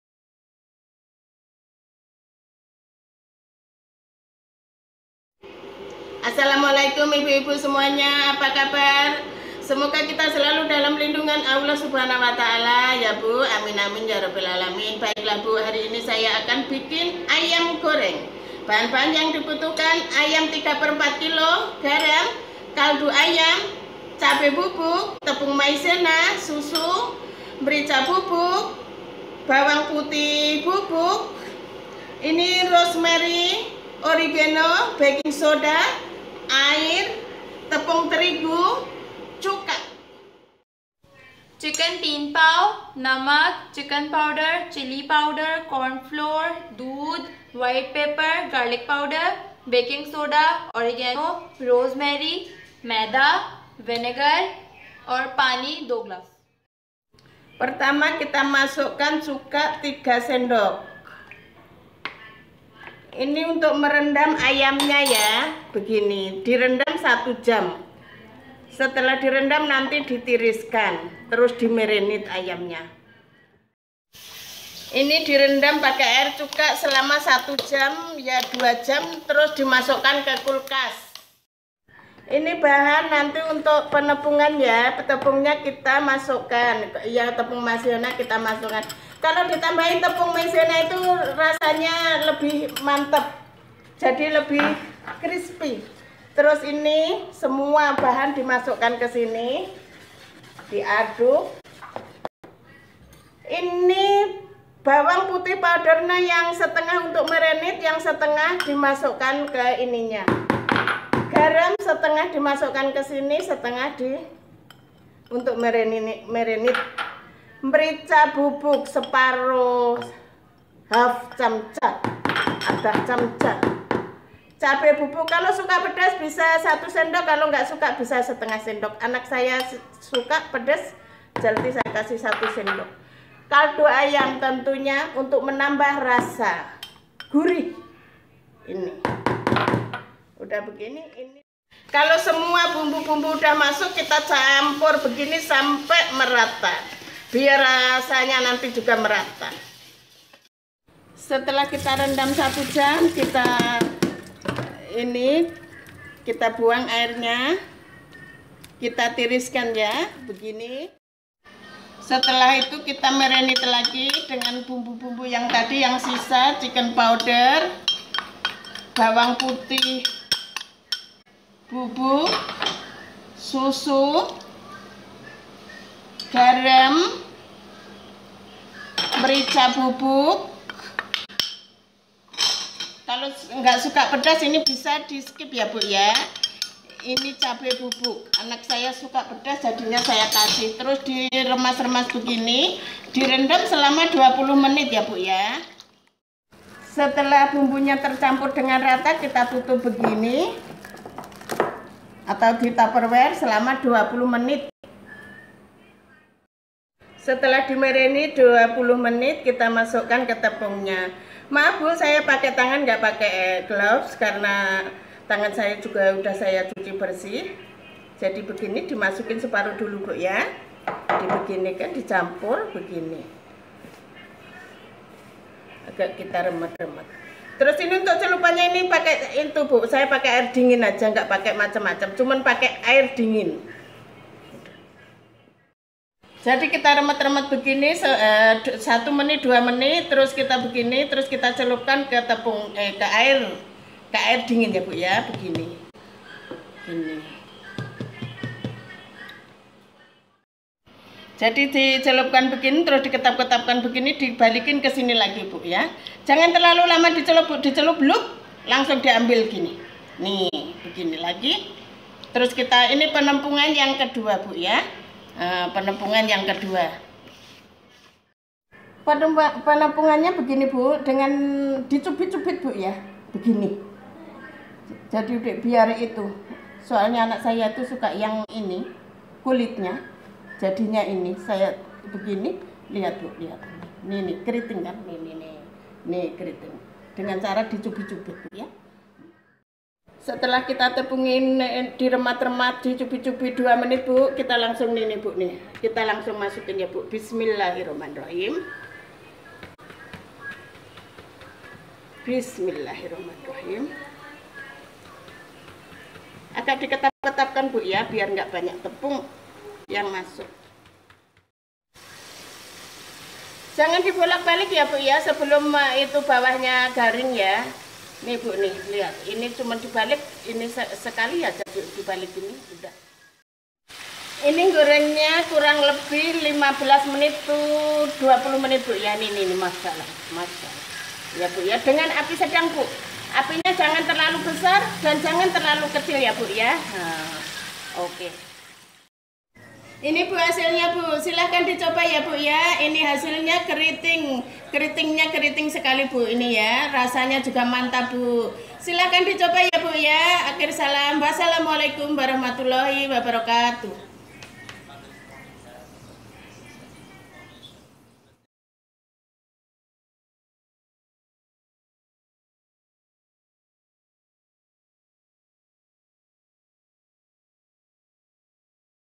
Cara Bikin Ayam Goreng Yang Kriuk Dan Kribo ( Easy Crispy Fried Chicken Recipe ) 🍗
Ingredients:
750g chicken
2tsp Salt
2tsp Chicken powder
2 tsp Chilli powder
2tsp White pepper
3 tbsp Milk
3 tbsp Cornflour
2 tsp Garlic powder
1/2 tsp Origano
1/2 tsp Rosemary
1tsp Baking soda
3tbsp Vinegar
1kg All purpose flour
2glass Water

Preparation:

• Add 3tbsp of vinegar in 2 galss of water.
• Soak 750g of chicken in it and put it in refrigerator for 1 hour.
• Take 1kg All purpose flour. Add in 3tbsp cornflour, 1/2tsp Origano, 1/2tsp rosemary, 1tsp baking soda, 1tsp garlic powder, 1tsp salt, 1tsp white pepper, 1tsp chilli powder and 1tsp chicken powder and mix it thoroughly.
• Here your coating material is ready.
• After an hour drain off water from soaked chicken.
• Marinate the chicken by adding 1tsp chicken powder, 1tsp chilli powder, 1tsp garlic powder, 3tbsp milk, 1tsp salt and 1tsp white pepper. Mix them well.
• Cover the chicken with plastic wrapper. Set aside  for 20 minutes.
• Coat the marinated chicken by dipping it in all purpose flour mixed with seasonings.
• Take cold water and dip the coated chicken in it.
• Again coat the chicken with the coating material.
• Pour oil to a heavy buttom pan and heat it.
• Shake off the excess flour from the chicken. Drop one after the other to hot oil.
• Fry them on medium high heat till golden.
• Keep stirring often until done.
• Serve fried chicken hot with ketchup or your favourite dip.